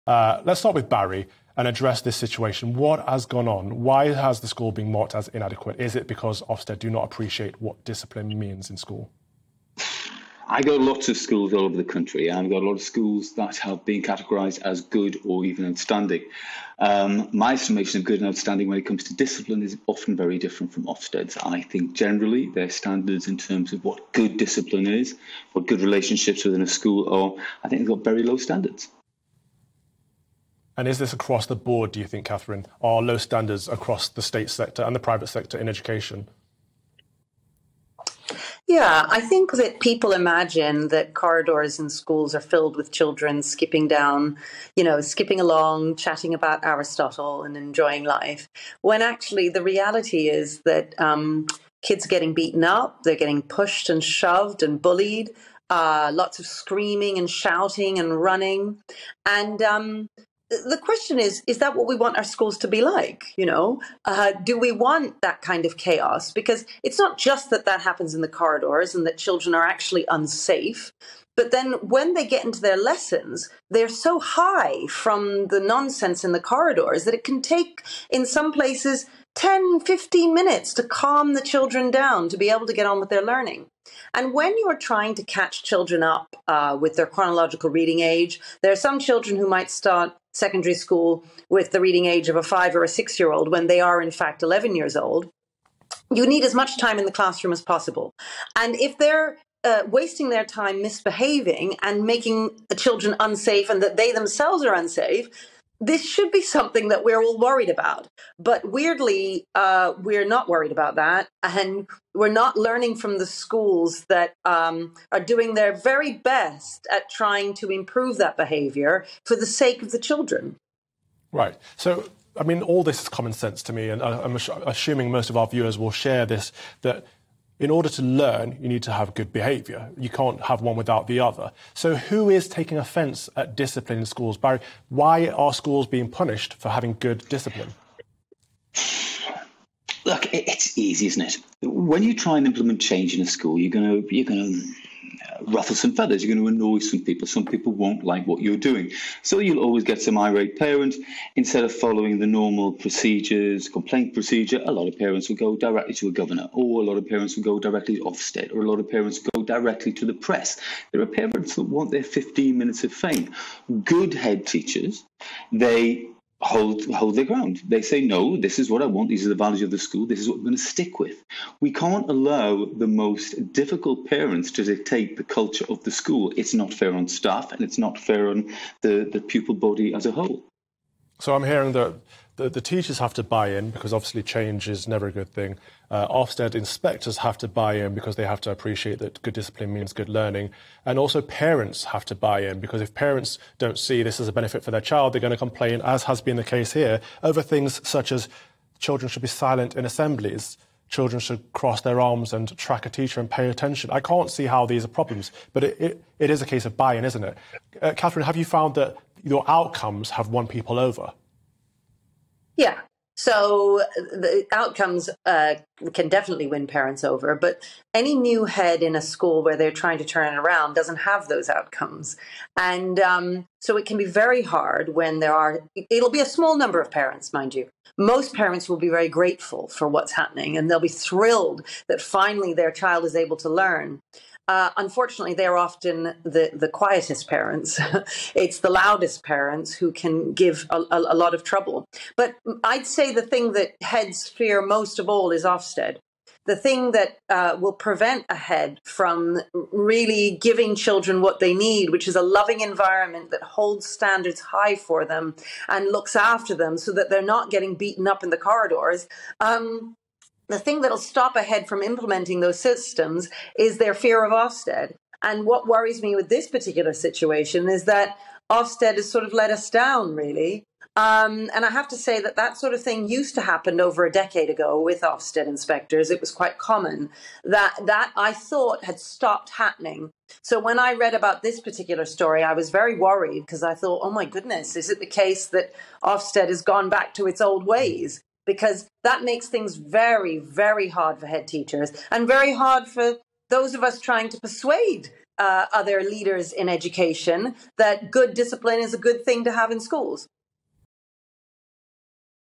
Discipline in schools | Ofsted's 'standards' of good discipline 'are very low': Education Consultant
Education Consultant Barry Smith and Founder and Headmistress of Michaela Community School, Katharine Birbalsingh, discuss discipline in schools.